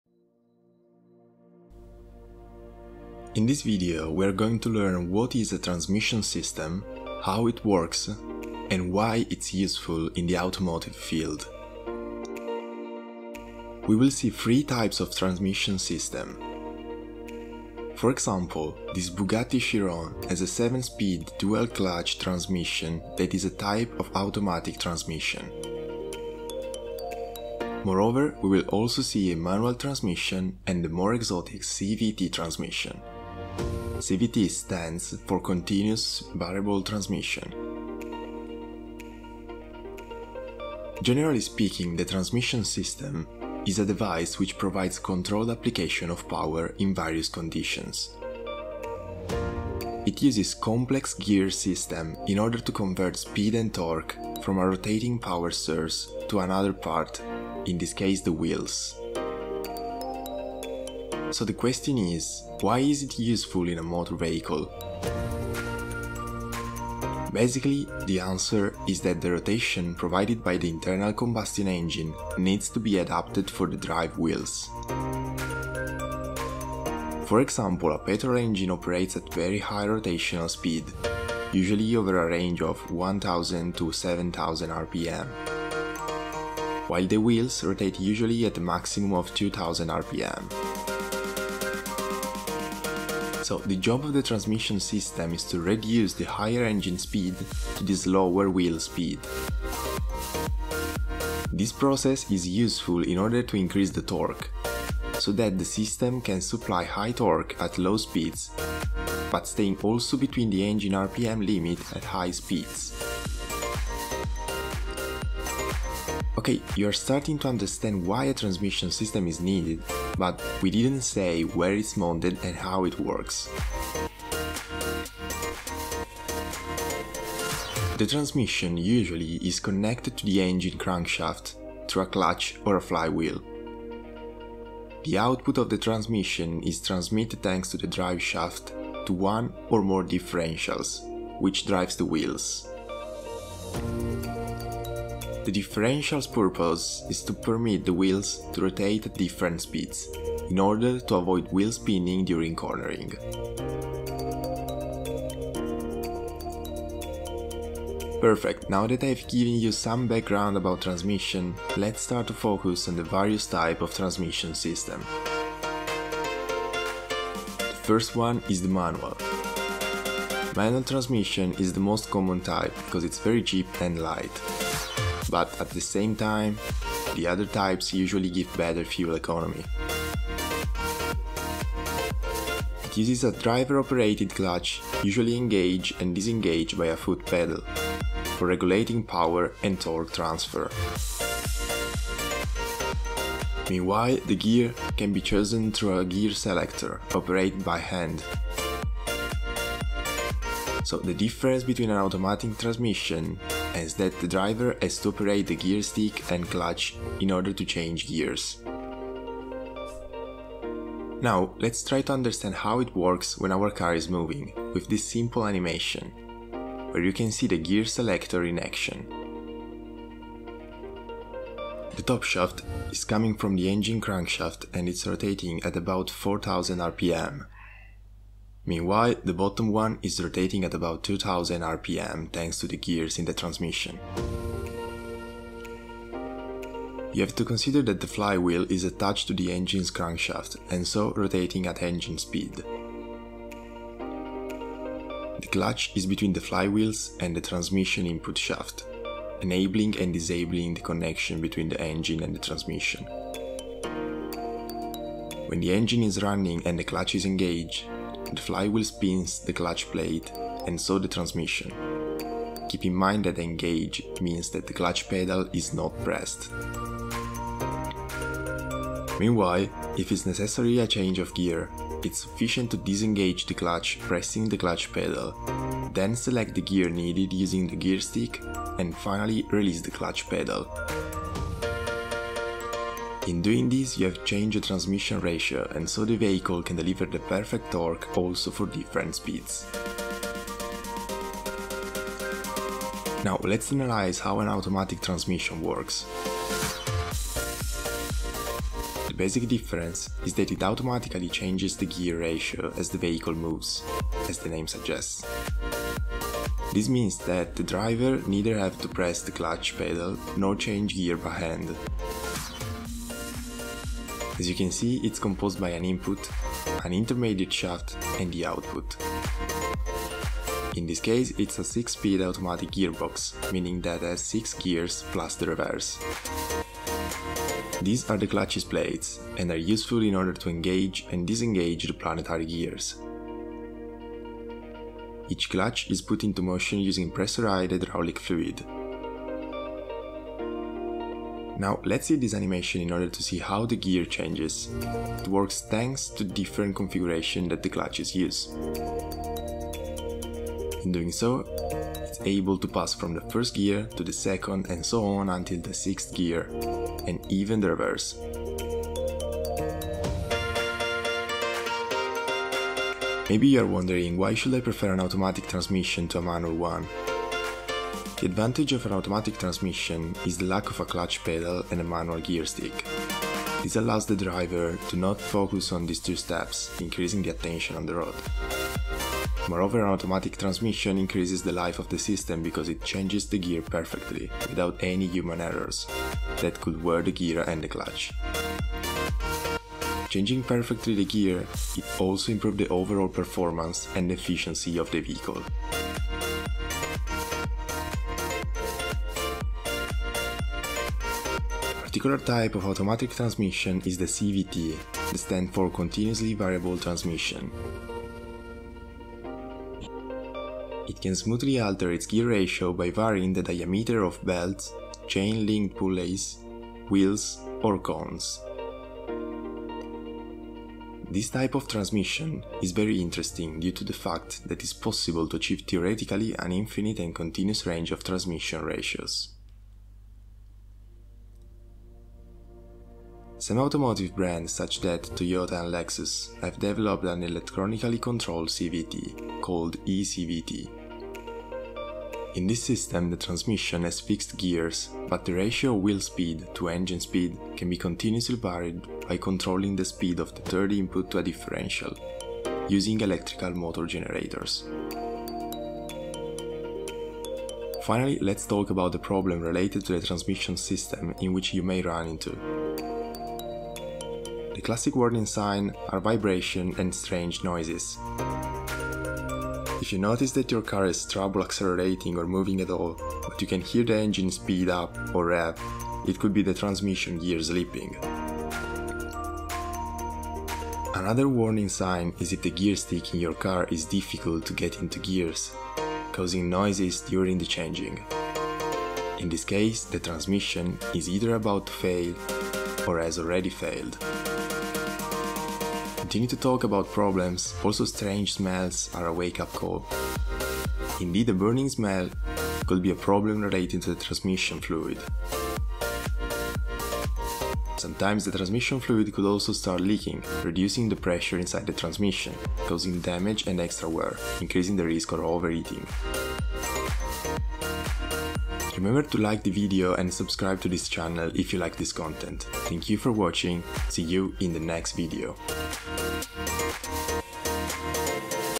Transmission System and it's Working
A video explaining what is Transmission system ,it's working and finally what are the most important types in Transmission System of automobile sector.